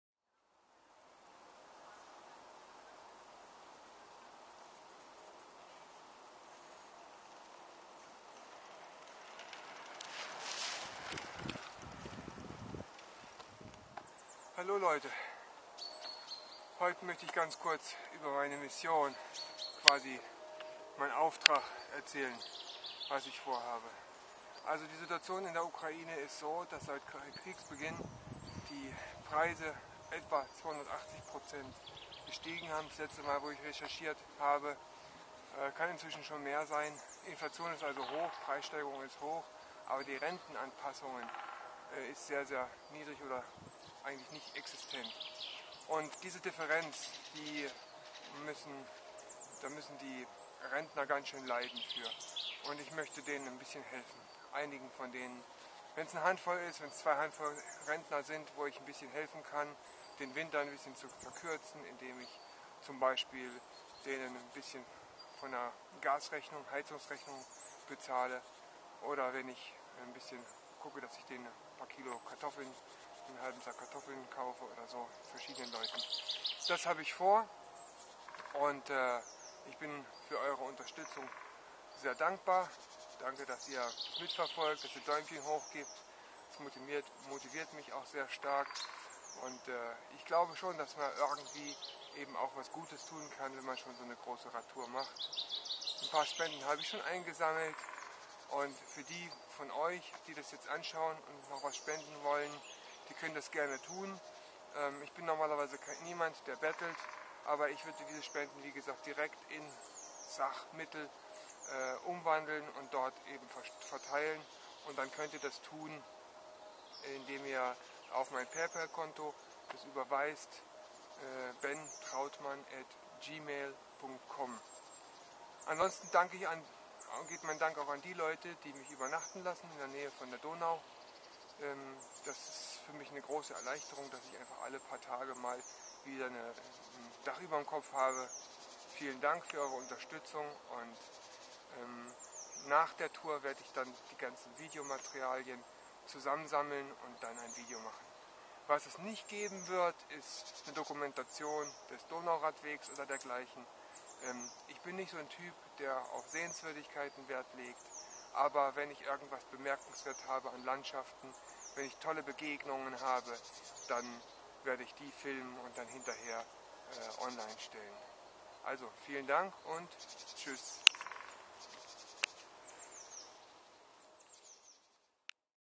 OmaRadTour: Die Mission
In diesem Plausch rede ich über meine Mission, meinen Auftrag, den ich mir gestellt habe mit der OmaRadTour: Es geht um eine Benefizradtour, für einen guten Zweck, und zwar, möglichst vielen Rentnern in der Ukraine zu helfen

- Durch die Preissteigerungen seit Kriegsbeginn in der Ukaine bei gleichbleibenden niedrigen Renten müssen die meisten Rentner schon bei den Nahrungsmitteln und der Gasheizung sparen. Spenden, die ich bekommen habe, werde ich umwandeln in Lebensmitelspenden (Kartoffeln) und in Wärme (Gasrechnung für die Heizung). Die Beträge, die während der Tour eingehen, kann ich noch diesen März verteilen. Man kann mir gebührenfrei Spenden hier zukommen lassen: (Dabei fungiere ich als Überbringer und Voluntär, so als ob der Spender die Spende selbst vor Ort entrichtet hätte).

Was den Videorahmen sprengen würde, aber dennoch erwähnenswert ist:
- Was nach der Tour eingeht, wird für den nächsten Winter 2017/18 verwendet werden. Aus Erfahrungen in der Arbeit mit humanitärer Hilfe von 1996 bis 1998 weiß ich, dass Hilfe nur dann effektiv ankommt, wenn sie

a) direkt geleistet wird, d.h. ohne evtl. korrupte Organisationen und Mittelmänner, am besten vom Spender zu 100% direkt zu Empfängern. Auch bei der Stückelung sollte in Summe 100% der Spende an die Empfänger gehen. Die Verteilaktion wird auf Volutärbasis gemacht: Ich nehme keinen Cent, sondern gebe alles weiter. Das ist mir wichtig. Wer mich persönlich mit einer Übernachtungsmöglichkeit oder Proviant unterstützen will, kann dies ausdrücklich so benennen. Ansonsten bestreite ich die Reise auf eigene Kosten.

b) lokal verteilt wird, d.h. ortsansäßige mir bekannte Leute kennen und beobachten die Situation langfristig. Dabei wird die Gefahr der Fehlverteilung wird minimiert. Es ist nicht auszuschließen, das jemand, der noch dürftiger ist, dieses Mal übersehen wird, aber es wird minimiert. Meine Schwiegereltern sind vor Ort.

c) zeitnah wie möglich, d.h. wenn heute Not gelindert werden kann, dann heute helfen und nicht auf morgen aufzuschieben. (Vergleiche dazu Sprüche Kapitel 3 Vers 27).

Noch ein Prinzip ist mir nach (leider Erfahrung mit korrupten OrganisatorenI wichtig ist:
d) Möglichst allen Menschen gutes zu tun, aber zuerst den Gläubigen, d.h. diese nicht zu übergehen, nur weil sie gläubig sind und daher das Leid stärker ertragen können sollten (Vergleiche dazu den Brief an die Galater in Kapitel 6 Vers 10).

Ergänzung nach Lektüre der bisher eingegangenen Kommentare: Das letzte Prinzip ist mir wichtig zu erwähnen, da in der Vergangenheit notleidende Menschen aus der örtlichen Glaubensgemeinde übersehen worden sind, weil man mit der Quantität der Hilfe "nach außen" irgendwie Eindruck erwecken oder bemerkt werden wollte. Meine Hilfe ist so unpolitisch motiviert wie sie von den Spendern selber gegeben worden ist. Ich verstehe mich lediglich als Verteiler vor Ort, ein dienender Überbringer.